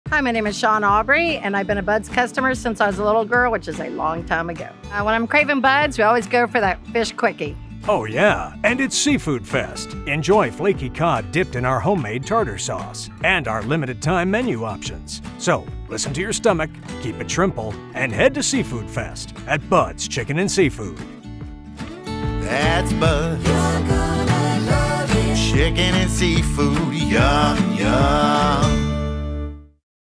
Seafood Fest 30 Second
OH YEAH, AND IT’S SEAFOOD FEST! ENJOY FLAKEY COD DIPPED IN OUR HOMEMADE TARTAR SAUCE. AND OUR LIMITED TIME MENU OPTIONS! SO, LISTEN TO YOUR STOMACH. KEEP IT SHRIMPLE AND HEAD TO SEAFOOD FEST. AT BUD’S CHICKEN AND SEAFOOD!